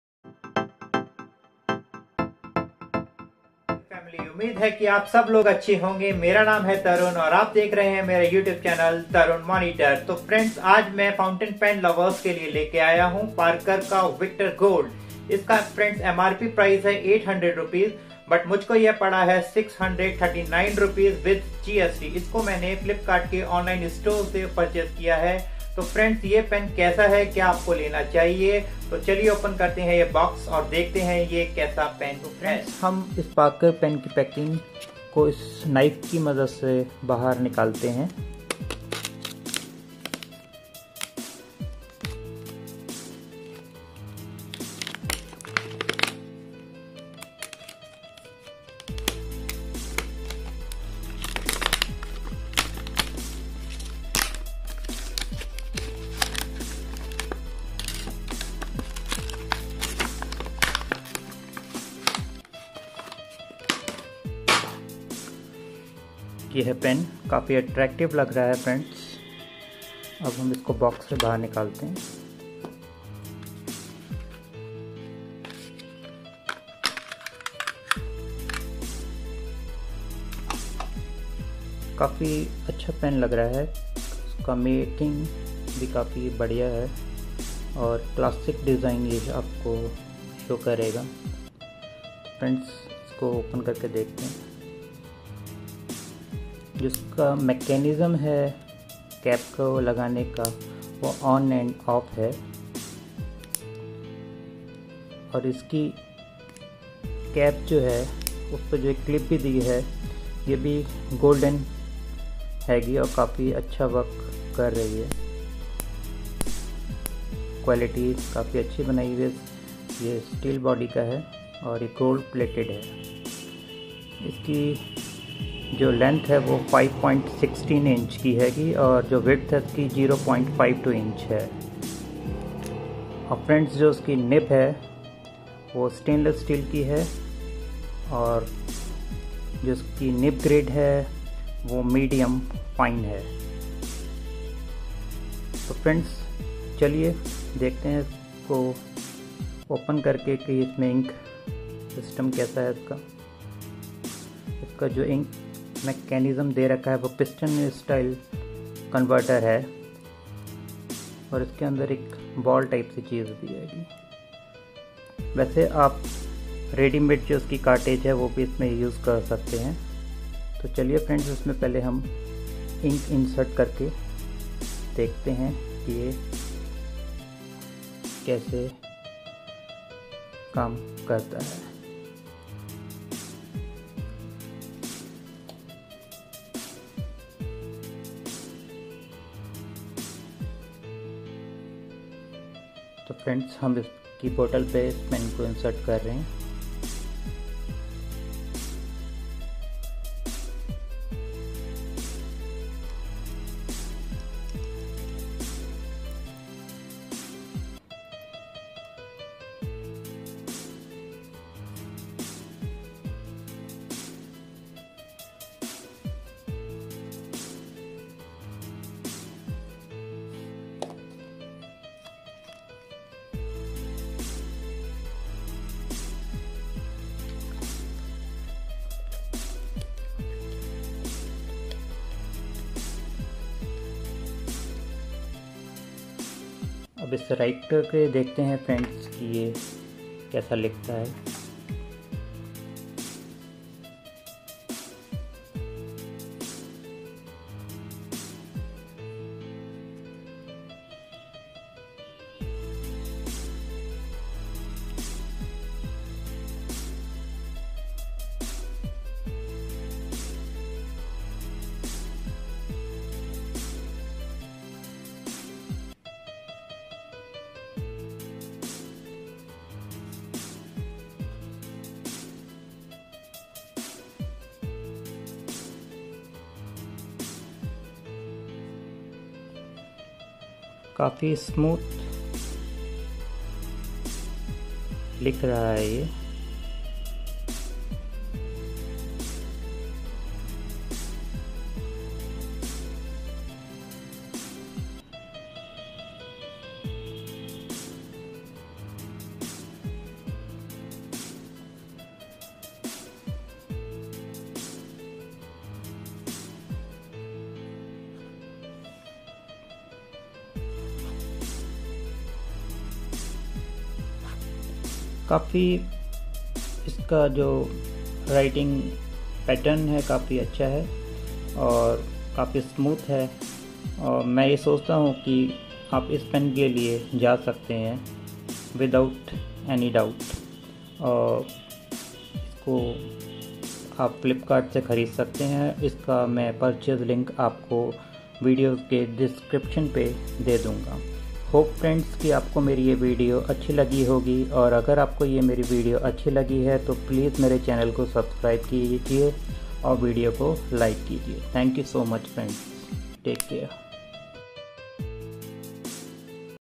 Parker Vector Gold ( Fountain pen) 🖋️📒
Parker 🖋️📒 really nice fountain pen brought in India by Luxor.. Really fine pen At price 639/- Ruppes.
purchase link as under :-

Parker Vector Gold (Gold Nib) Fountain Pen